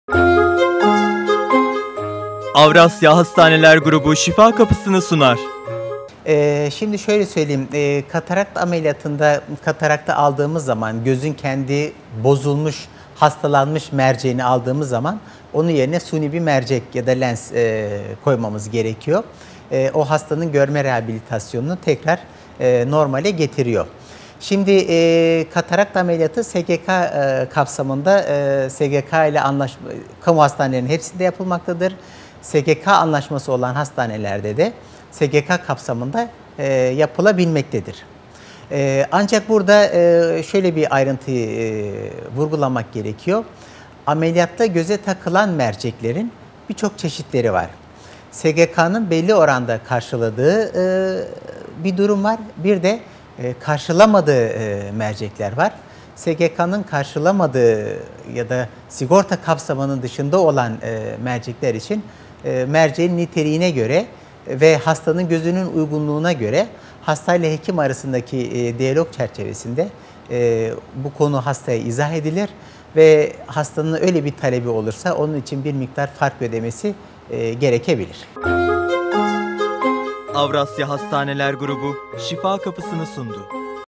Katarakt Ameliyatı
Avrasya Hastanesi Gaziosmanpaşa Göz Hastalıkları Uzmanı Op. Dr. Atilla Hacıbekiroğlu, katarakt ameliyatı hakkında bilgilendiriyor.